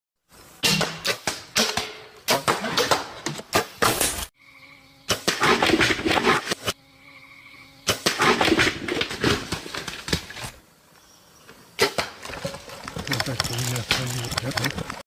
25 Huge bear shot compilation
We hope you enjoy our 30+ Huge bear shot compilation video with all archery only shots!
25 HUGE BEARS SHOT COMPILATION.
HUGE BEARS SHOT.
COMPILATION.
 BEARS SHOT COMPILATION.
HUGE BEARS.